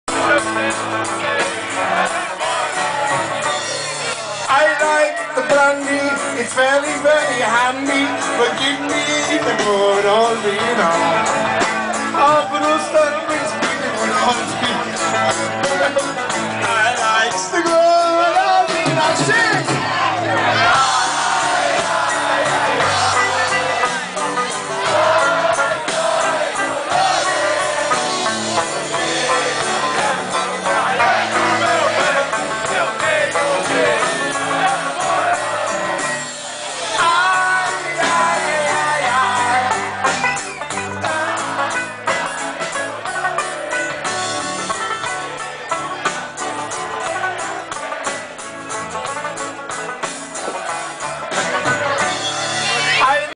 The Colonials - St. Patrick's Day 2011
The Colonials tocando no St. Patrick's day 2011 no The Irish Rover, pub irlandês em Madrid